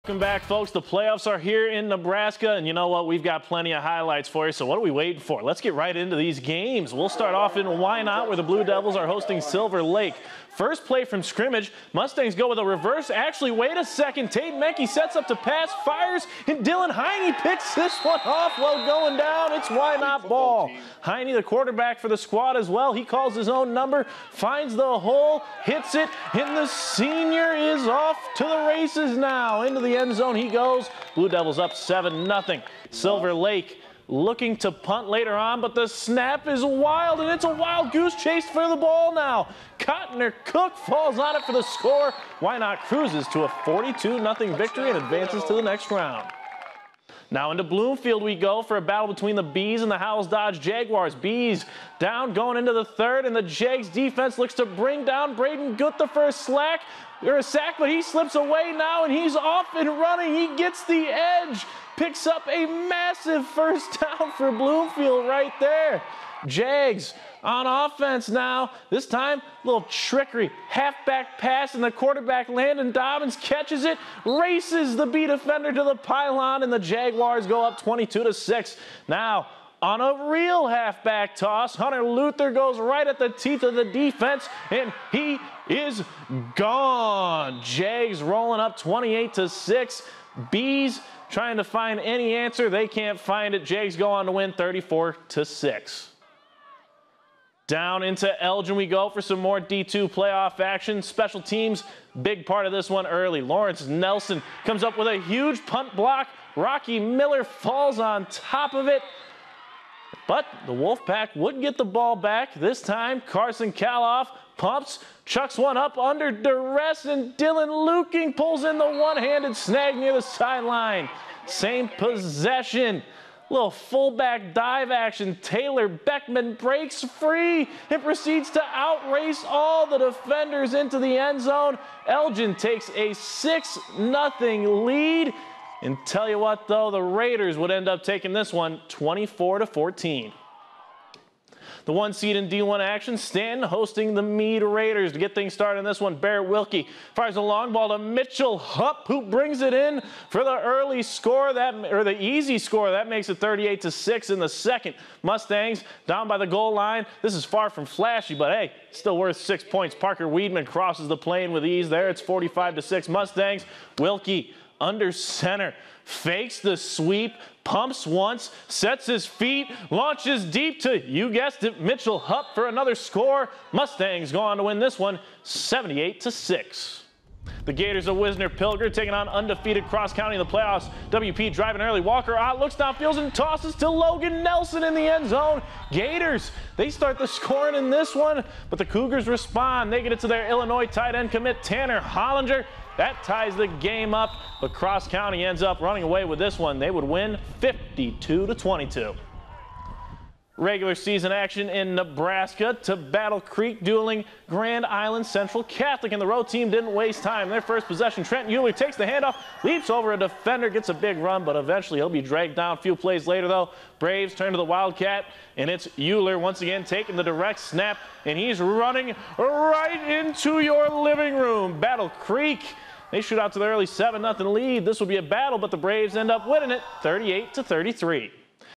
8-man playoffs begin in Nebraska, Battle Creek wraps up regular season with victory
8-man playoffs begin in Nebraska, Battle Creek wraps up regular season with victory